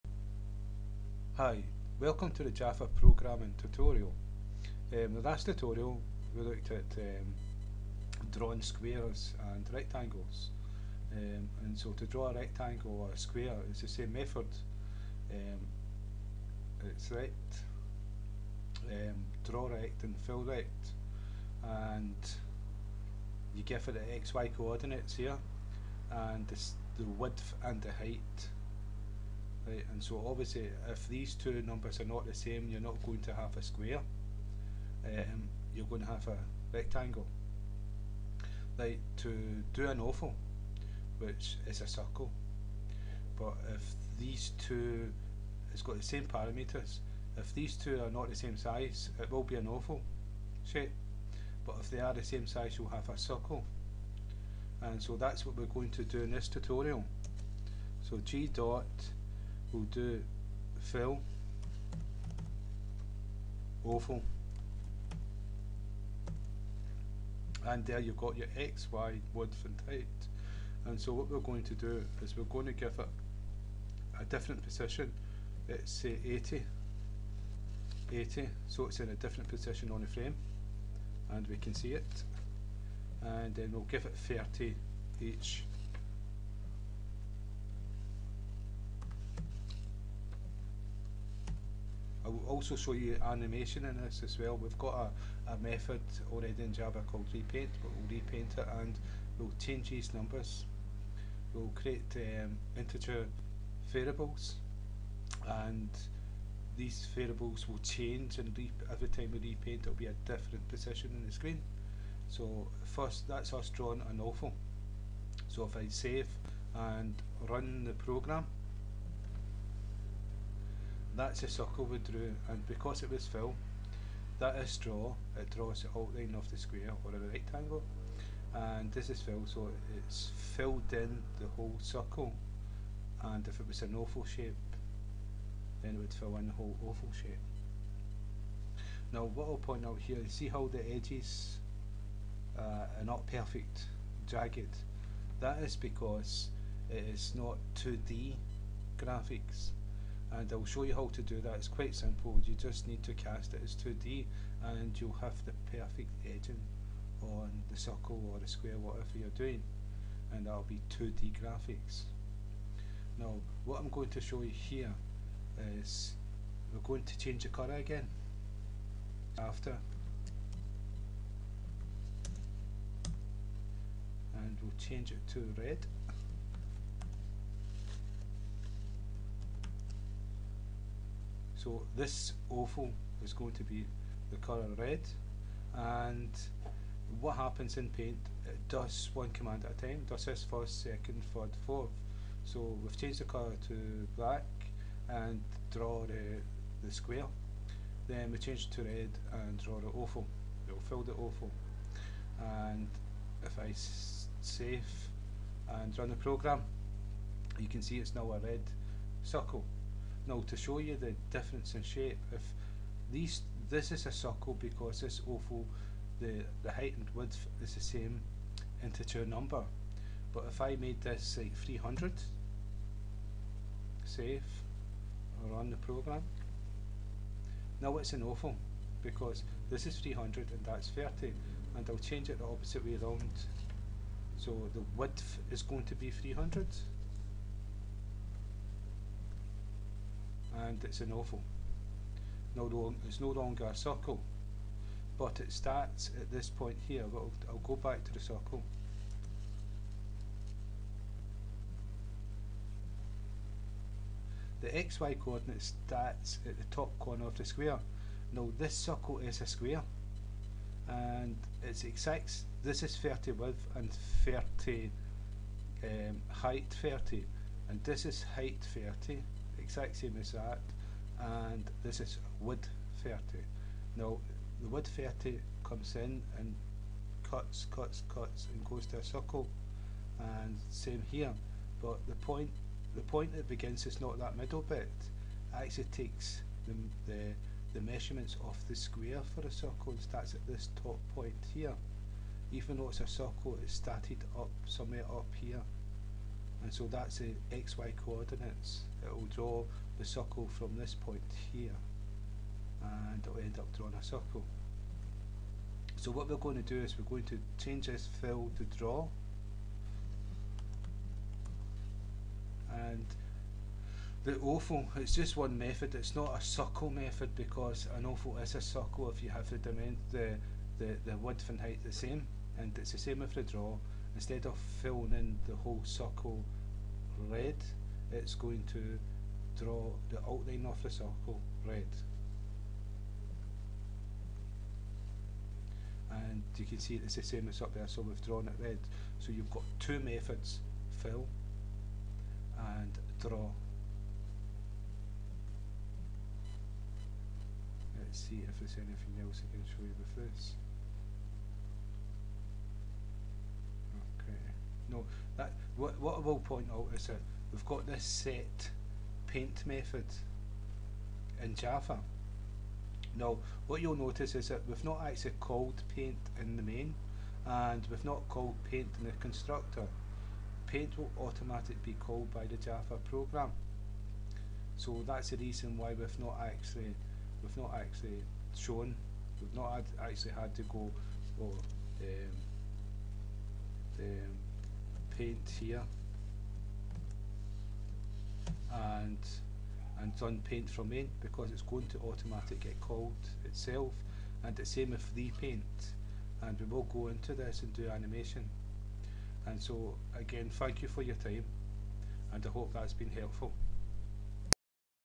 Java programming part 88 Graphics : Java fillOval and drawOval
.Java Programming tutorial, GUI, Graphical User Interface. this tutorial is teaching. How to use Java graphics for painting and drawing, Circles and Ovals.

import java.awt.Color;
import java.awt.Graphics;

import javax.swing.*;

public class GraphicsTut extends JFrame{

 public GraphicsTut(){
  this.setTitle("Remember g for Graphics");
  this.setSize(500,500);
  this.setDefaultCloseOperation(JFrame.EXIT_ON_CLOSE);
  this.setVisible(true);

 public void paint(Graphics g){
  g.setColor(Color.BLACK);
  g.drawRect(40, 40, 30, 30);
  g.setColor(Color.RED);
  g.drawOval(80, 80, 30, 30);

  new GraphicsTut();